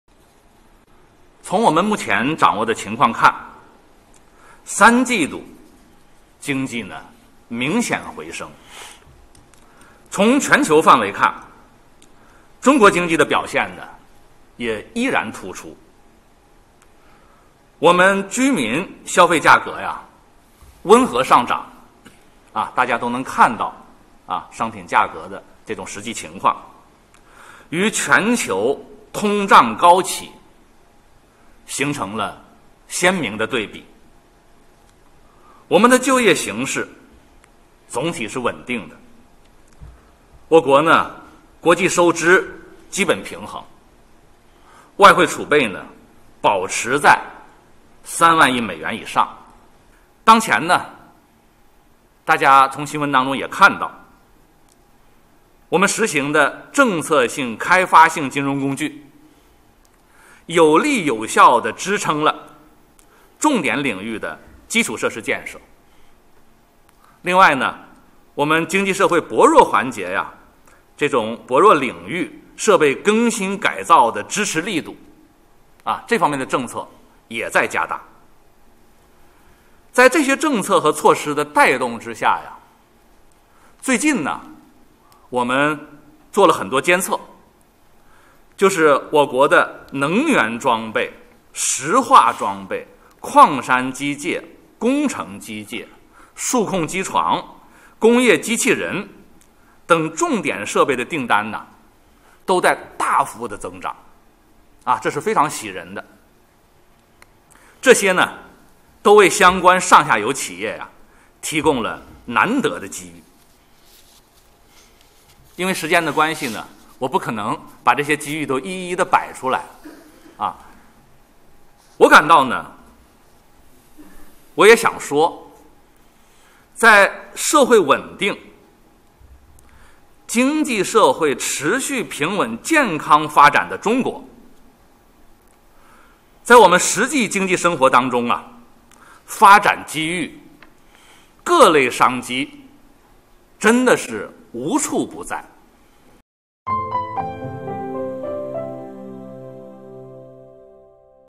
【聚焦二十大】国家发改委：三季度经济明显回升，多个领域订单大幅增长
10月17日上午， 二十大新闻中心举行第一场记者招待会。介绍贯彻新发展理念、构建新发展格局、推动高质量发展，以中国式现代化全面推进中华民族伟大复兴有关情况，并回答记者提问。
国家发展和改革委员会党组成员、副主任赵辰昕表示，从目前掌握情况看，中国三季度经济明显回升。从全球范围看，中国经济的表现依然突出。“我们的居民消费价格温和上涨，与全球通胀高企形成了鲜明对比；我们的就业形势总体稳定；国际收支基本平衡，外汇储备保持在3万亿美元以上。”